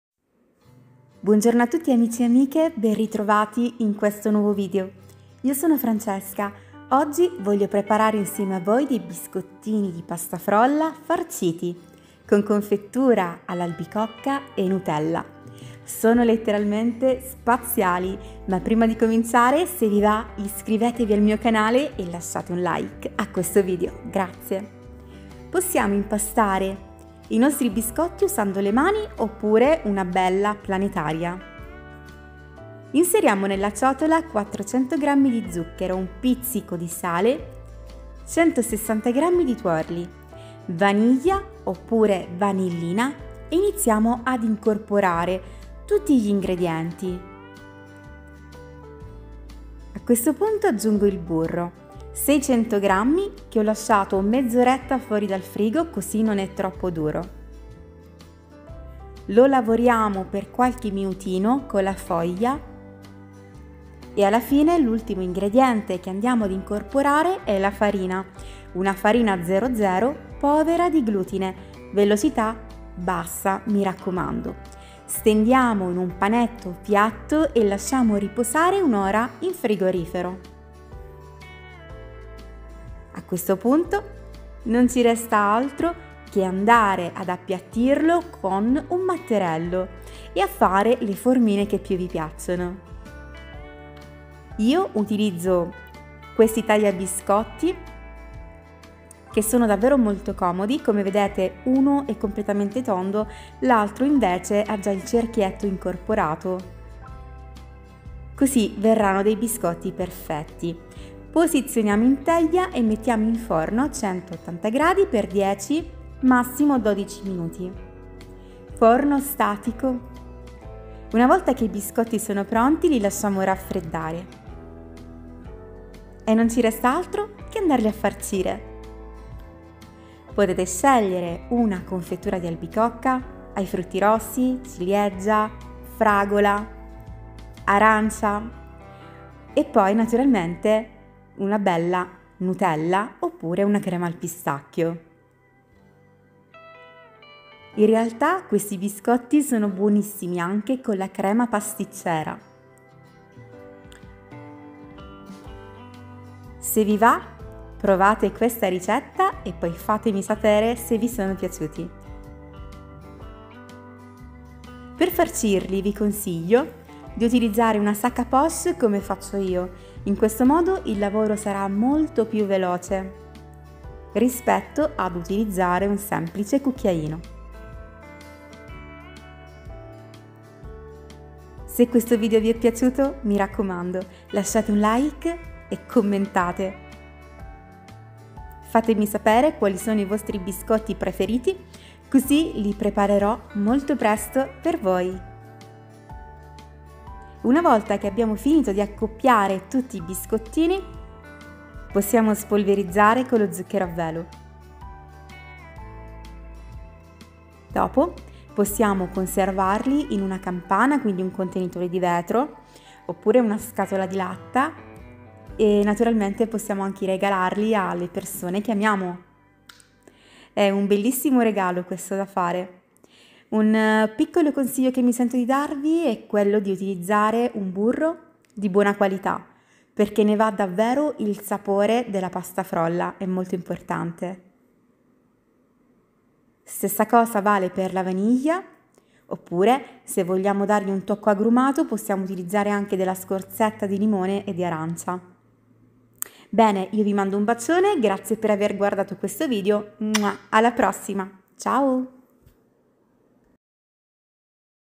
BISCOTTINI DI PASTA FROLLA MORBIDI FARCITI CON MARMELLATA E NUTELLA DELIZIOSI FACILI E VELOCI
Occhi di bue : biscottini morbidi di pasta frolla farciti con marmellata e nutella ! Prova questi sempli biscotti fatti in casa e te ne innamorerai!
Impossibile resistere! Uno tira l'altro!

 Se la ricetta ti è piaciuta :
lascia un commento! Metti un like e condividila con gli amici 🙏
Ti ringrazio con tutto il mio cuore ❤️

‼️ In questo video:

Biscotti occhi di bue - Biscotti ripieni di marmellata e crema al cioccolato:

1 kg farina
600 g burro
400 g zucchero
160 g tuorlo
4 g sale
vaniglia

Recipe:

1 kg flour
600 g butter
400 g sugar
160 g egg yolks
4 g salt
vanilla

Jam, chocolate cream, powdered sugar